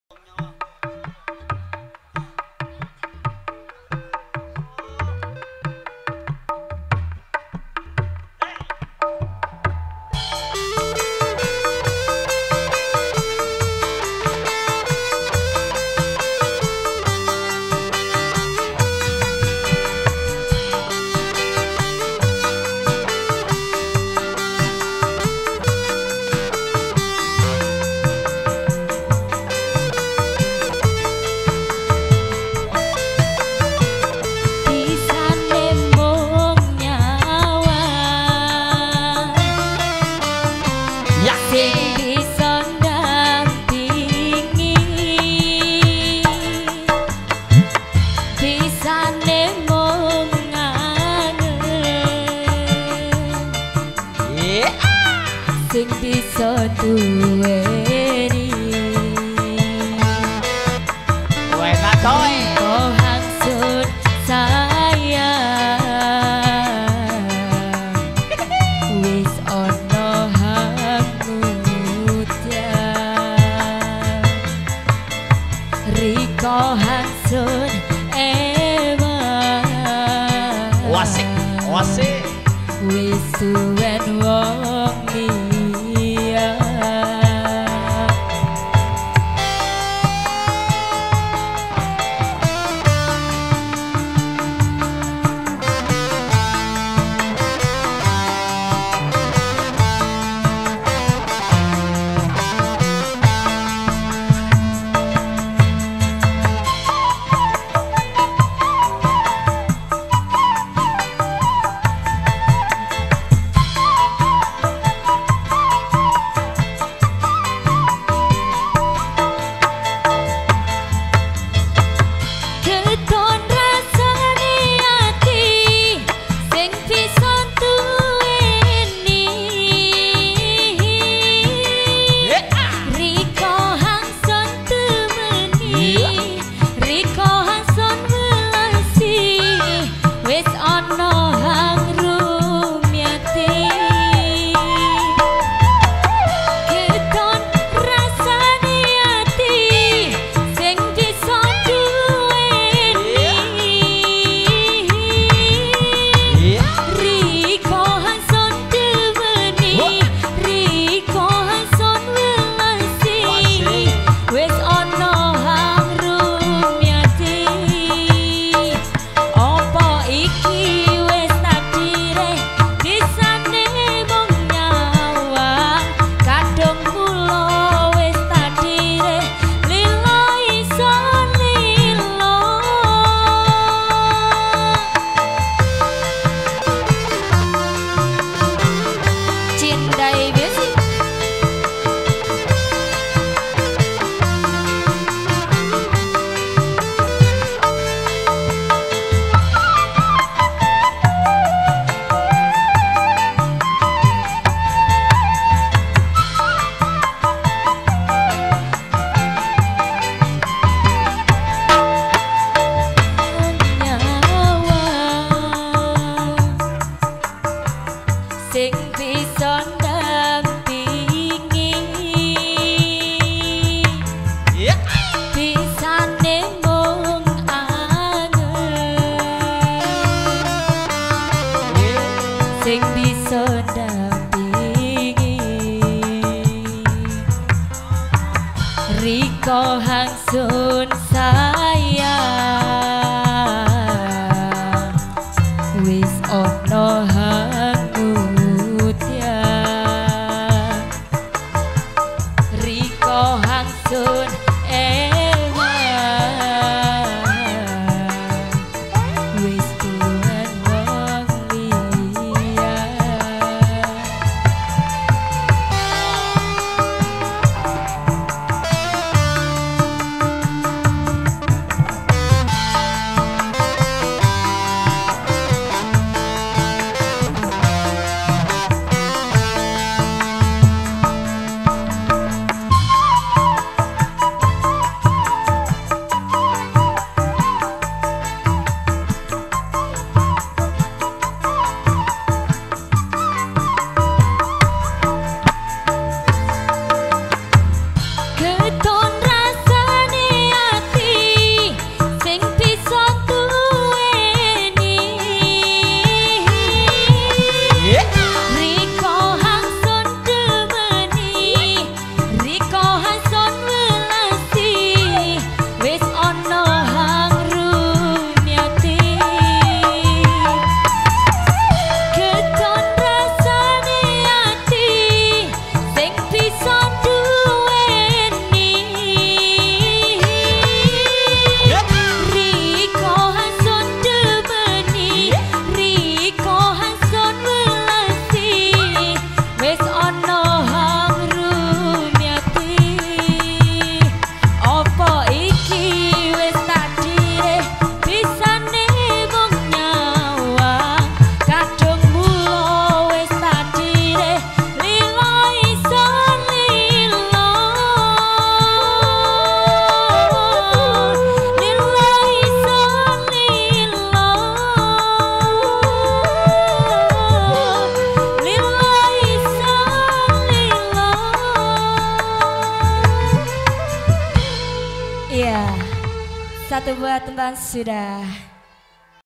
Artisnya Bisane Mung Nyawang
Artisnya yang ini cantik banget dan masih muda lagi punya bakat nyanyi sangat merdu suaranya